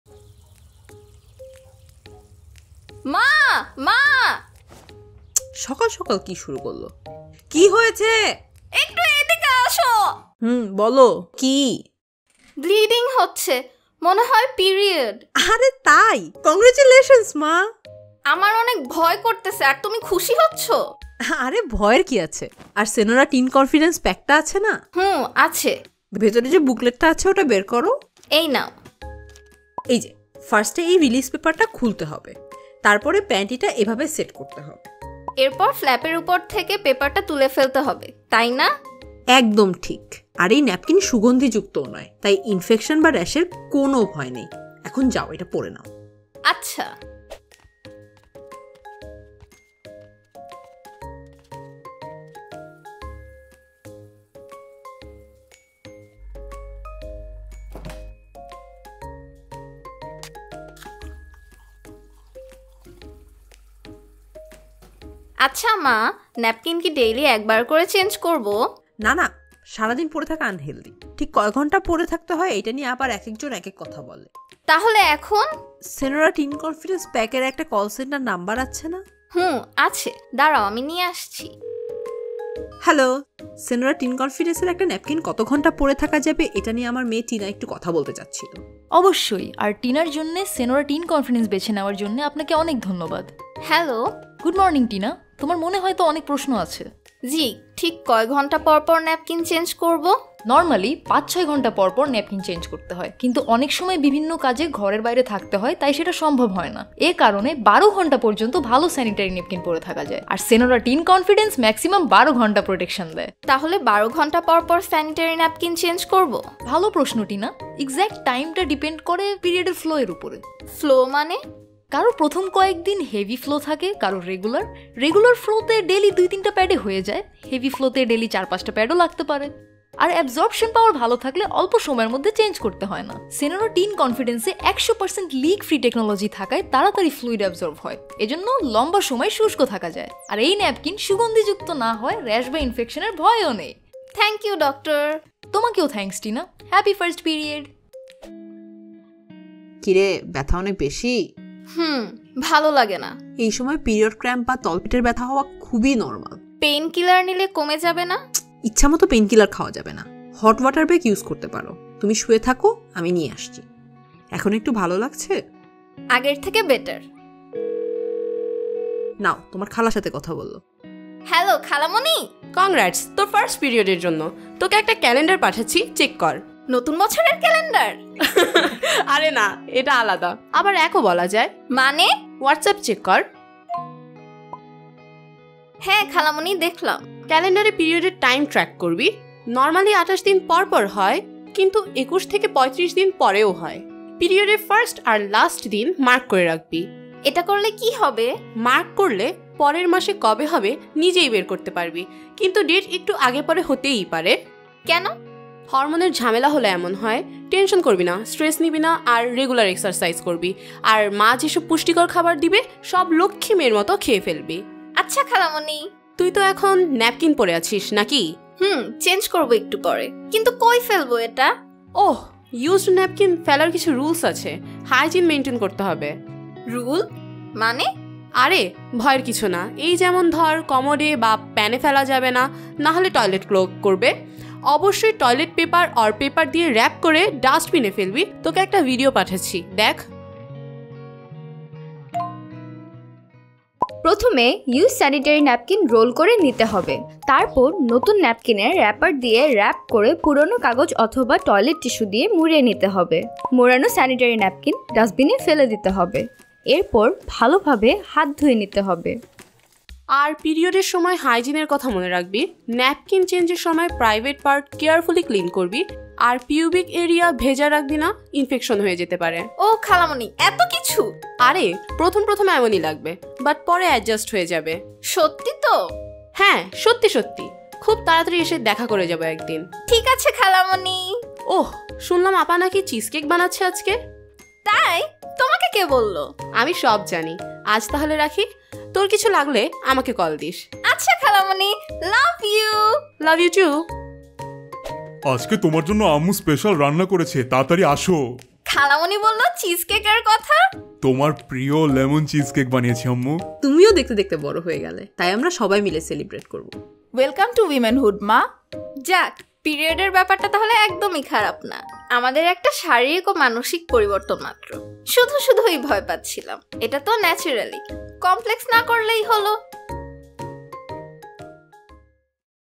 পিরিয়ডয়ের ভয় দূর করে, টিনার মতোই নিঃশঙ্ক চিত্তে প্রতিটি নারী এগিয়ে যাক কনফিডেন্সের সাথে।
টিনার প্রথম পিরিয়ড। ভয় দূর করে মা, খালামণি, ডক্টর সবাই তাকে কীভাবে কনফিডেন্স দিবে, জেনে নিন সম্পূর্ণ ভিডিও দেখে।